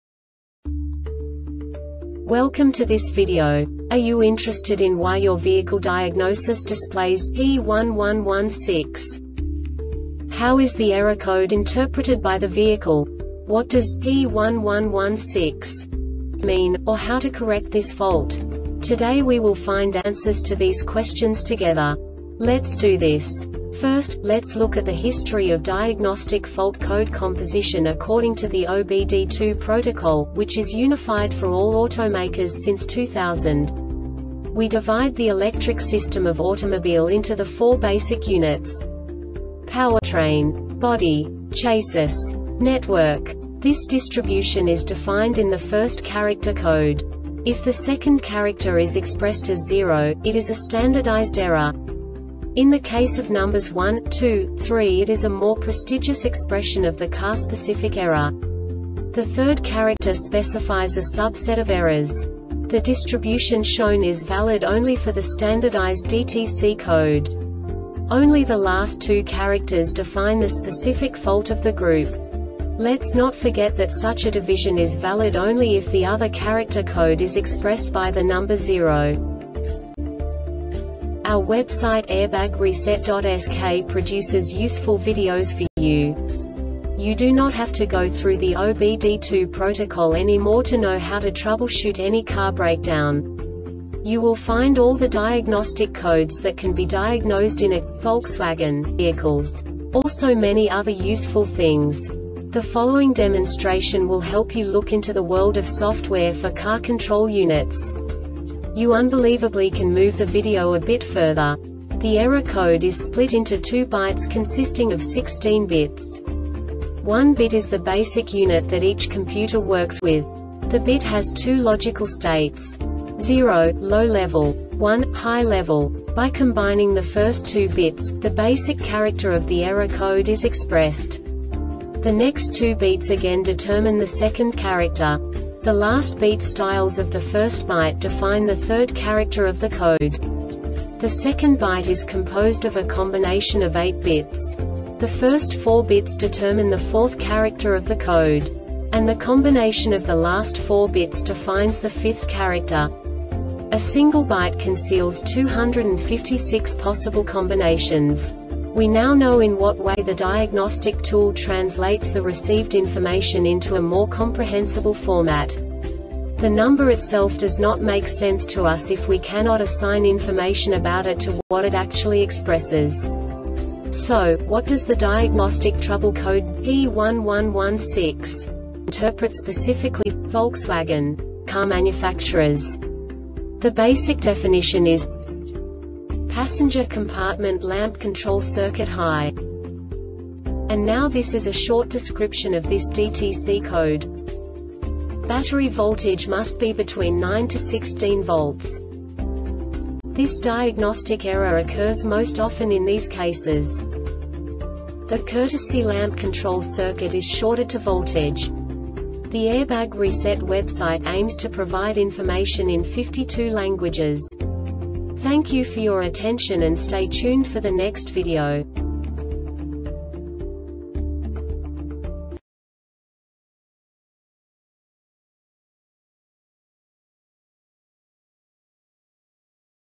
DTC Volkswagen P1116 Short Explanation
Contents:
0:21 Basic DTC analysis according to OBD2 protocol standard.
1:48 Insight into programming
2:58 A diagnostically understandable description of the Diagnostic Trouble Code
3:05 Basic error definition

Make:
Volkswagen
Code:
P1116
Definition:
Passenger Compartment Lamp Control Circuit High
Description:
Battery voltage must be between 9-16 volts.
Cause:
The courtesy lamp control circuit is shorted to voltage.